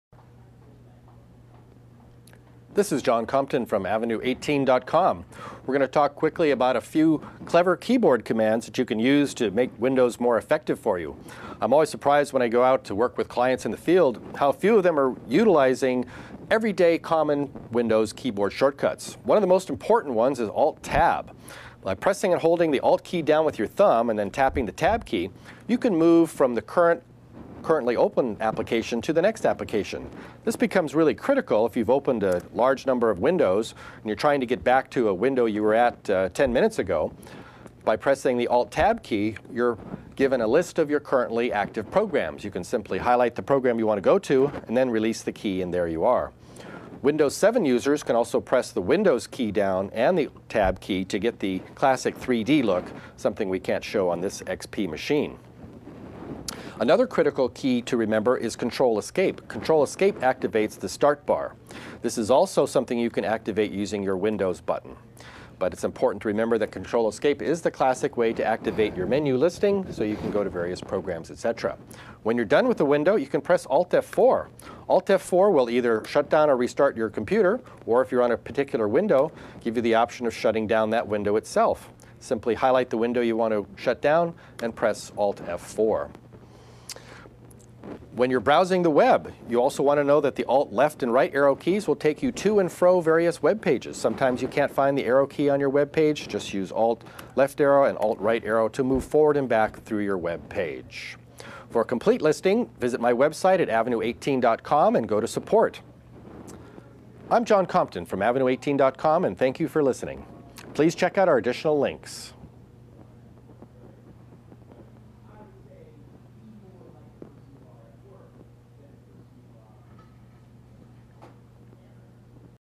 Computer Keyboard Shortcuts
There are computer keyboard shortcuts available for many of the different things that you do on thecomputer. These shortcuts can help you move from window to window, pull up the start menu on the computer and a number of other times saving things. Learning a few of the computer keyboard shortcuts that are available can help you optimize the time that you spend on some of these tasks.1
Thanks to John Compton from Avenue18.com

How to Make Your Computer Run Faster : How to Make Your Computer Run Faster